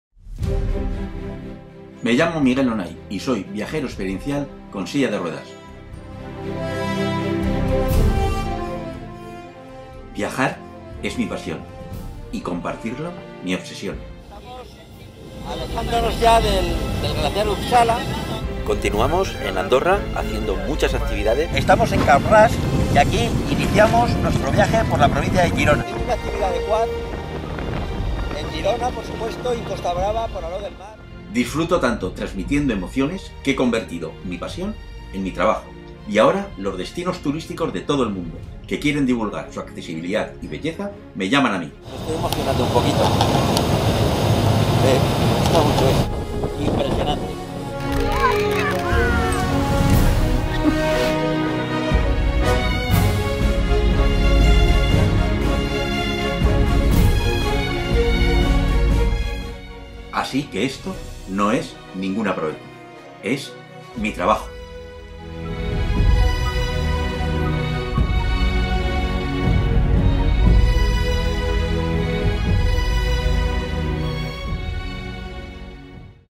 Qué es Viajeros sin límite
Vídeo resumen sobre el trabajo que realizamos con los destinos turísticos de todo el mundo para divulgar sus lugares accesibles y de gran belleza a través de un viajero con silla de ruedas.